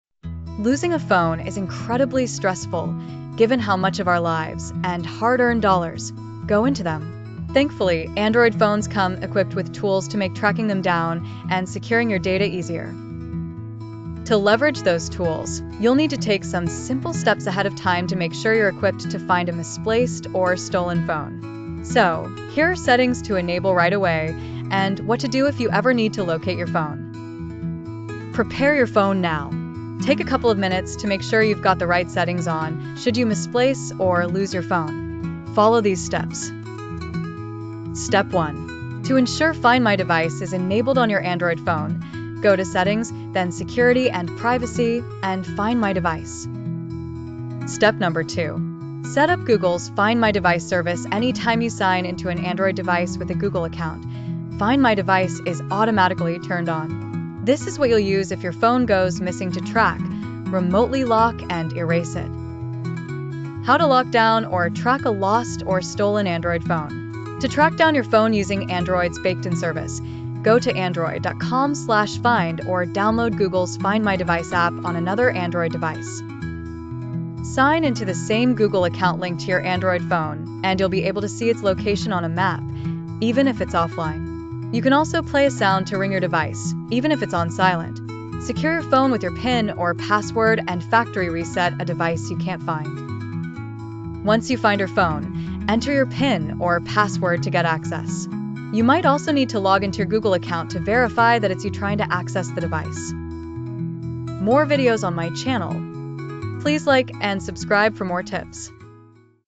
How To Find a Lost or Stolen Android Phone ? Easy Steps
This guide provides easy steps to locate a lost or stolen Android phone. It covers using Google's Find My Device feature, checking location history, and utilizing third-party apps. Additionally, it offers tips on securing your device and preventing future loss. With these methods, users can increase their chances of recovering their phone quickly and efficiently.